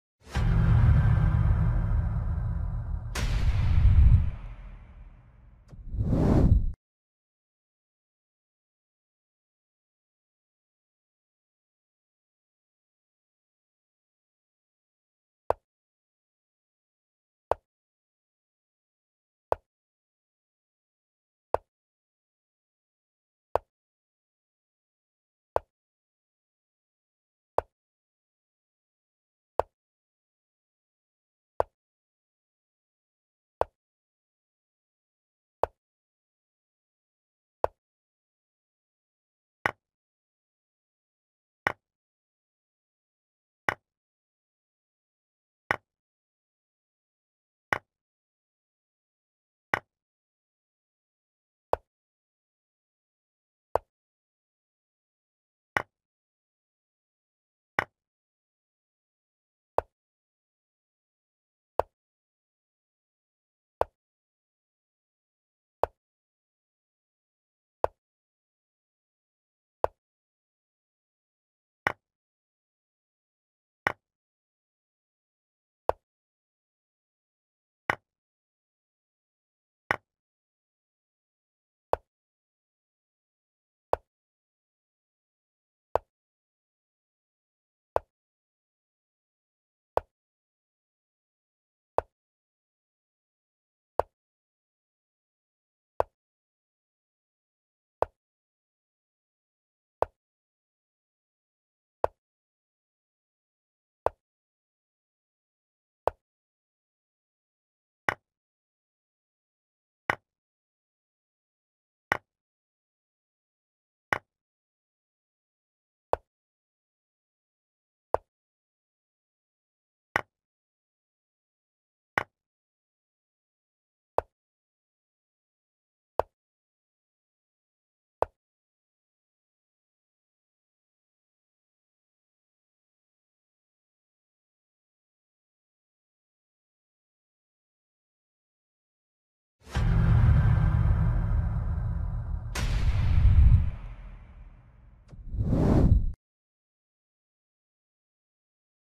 Steinitz - Gunsberg. World Chess Championship 1890–1891. Game 11
Wilhelm Steinitz - Isidor Gunsberg 1/2.
World Chess Championship 1890–1891. Game 11.
The third World Chess Championship was held in New York City in late 1890 and early 1891. Holder Wilhelm Steinitz (known as William Steinitz since his naturalisation as an American citizen) narrowly defeated his Hungarian challenger, Isidor Gunsberg.